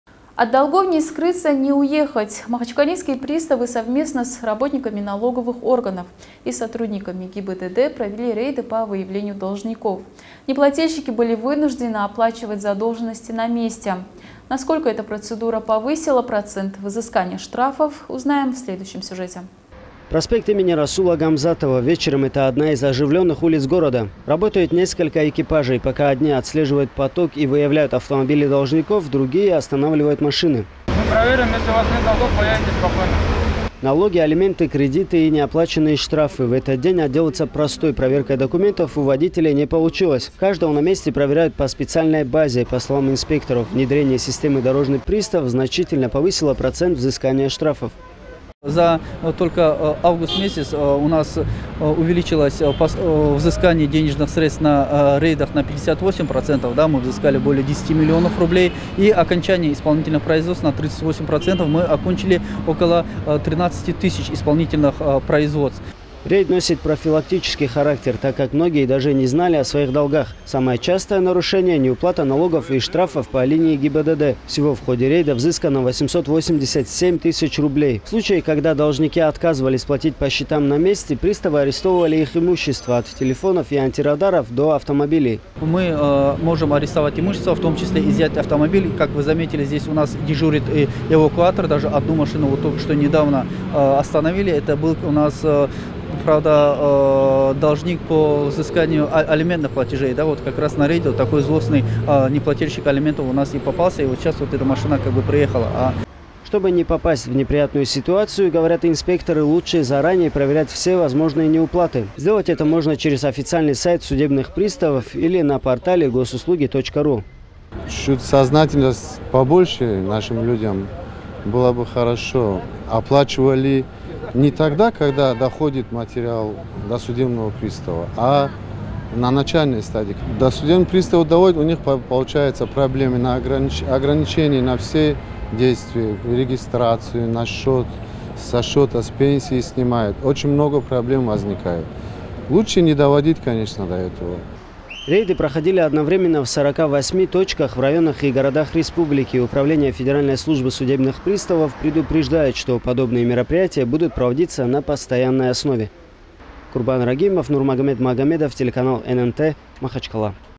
На улицах Махачкалы проводят рейды по выявлению неплательщиков штрафов
От долгов ни скрыться, ни уехать. Махачкалинские приставы совместно с работниками налоговых органов и сотрудниками ГИБДД провели рейды по выявлению должников. Неплательщики были вынуждены оплачивать задолженности на месте.

Насколько эта процедура повысила процент взыскания штрафов, узнаем в следующем сюжете.